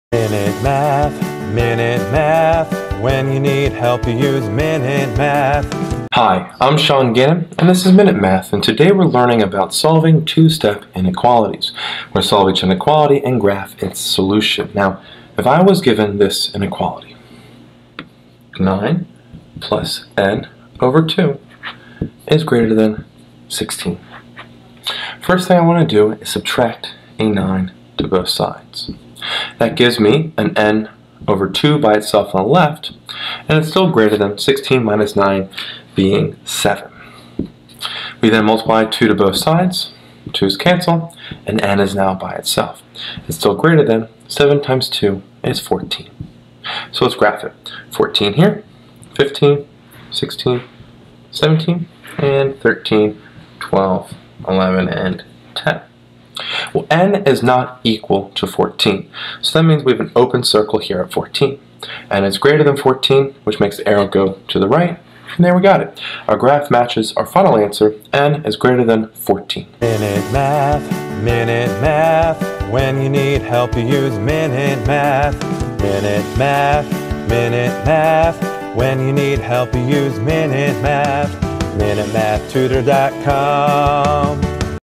Solve and Graph 9+n/2›16
In this math video lesson on Solving Two-Step Inequalities, I solve the inequality 9+n/2 greater than 16 and graph its solution.